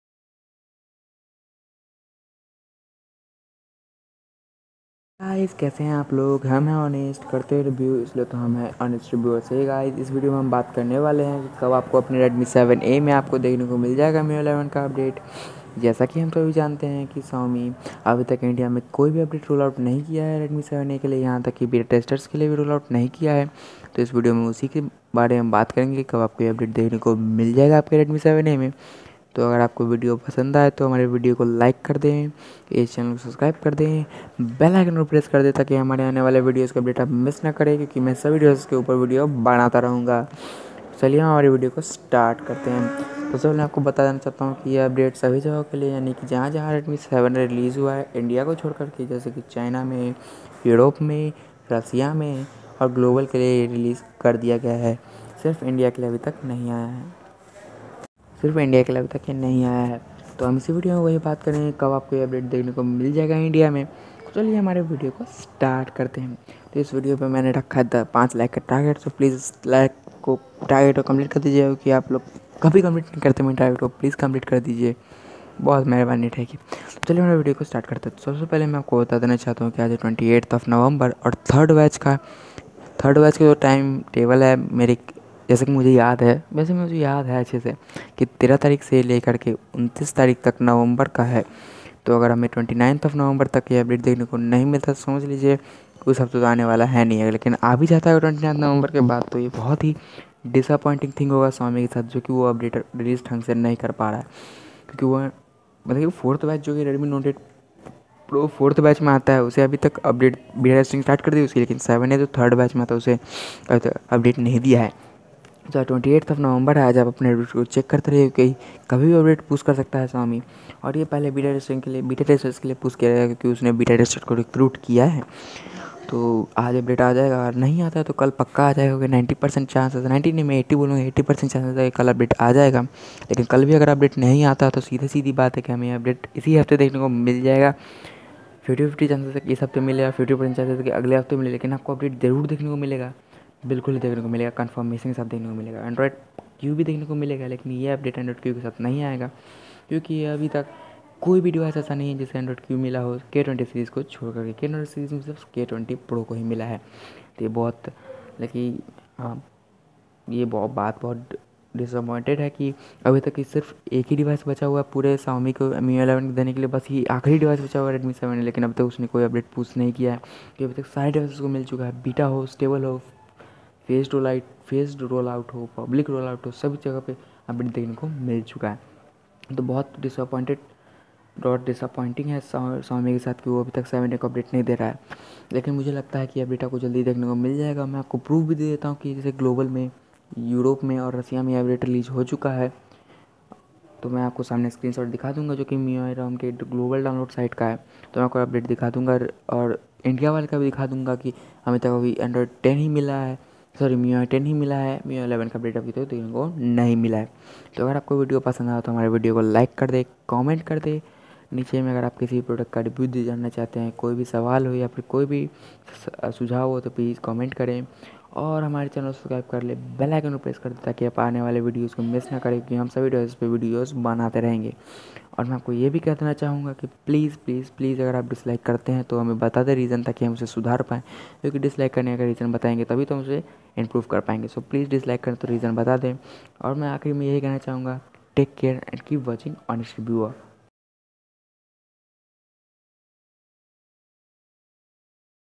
Redmi 7a miui 11 update release date in India full information update now
In this video we are telling you about the release date of miui 11 update for redmi 7a in India full information